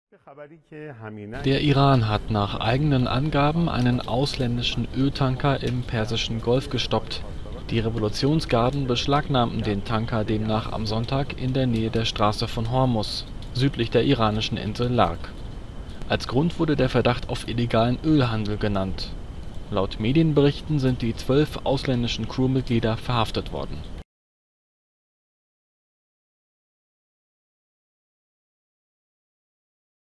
Iran stoppt Öltanker im Persischen Golf
Der Iran hat nach eigenen Angaben einen ausländischen Öltanker beschlagnahmt. Als Grund wurde der Verdacht auf illegalen Ölhandel genannt.…